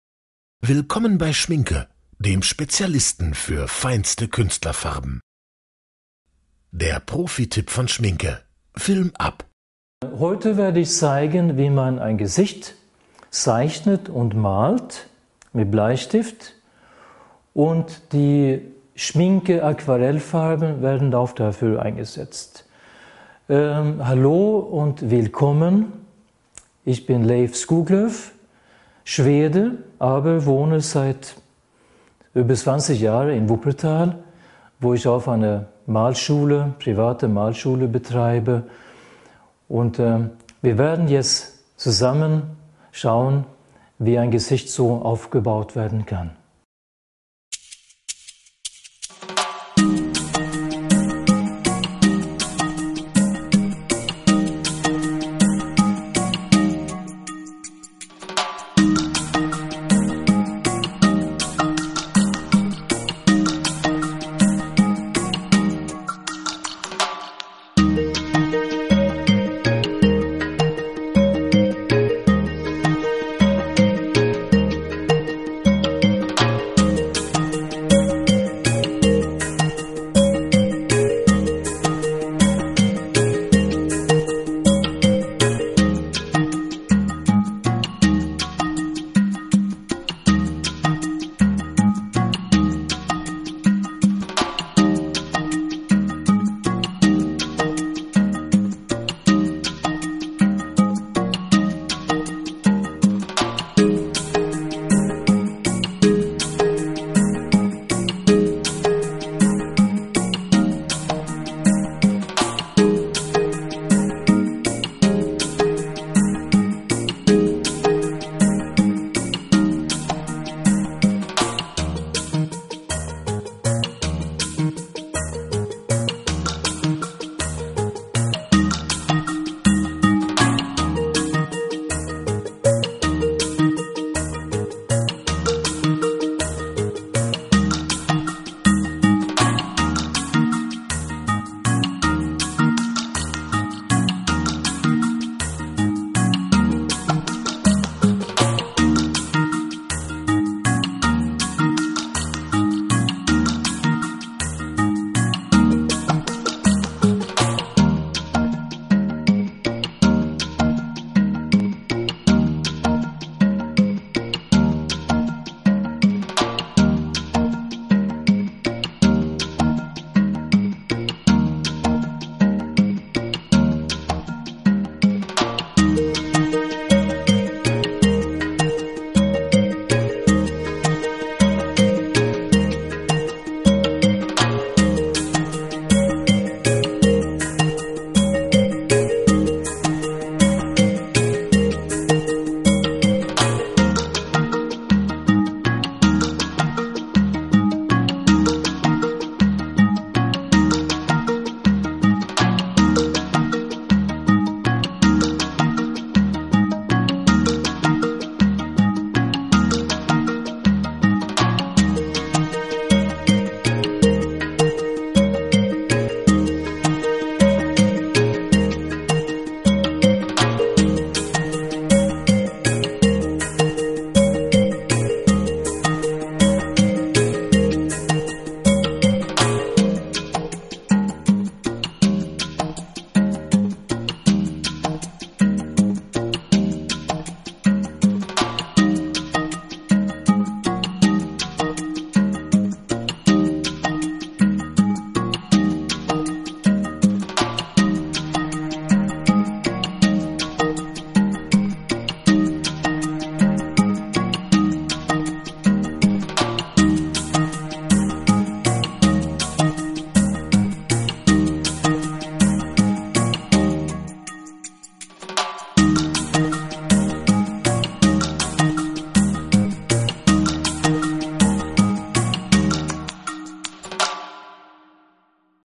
SCHMINCKE Profi-Tipp: Aquarell-Portrait
Portraits mit Schmincke Aquarellfarben - Tipps vom Profi

Anwendungsbeispiel zum Thema Aquarell-Portrait. Produkt: HORADAM® AQUARELL.

Wir freuen uns natürlich sehr über Kommentare oder ein Like! DANKE!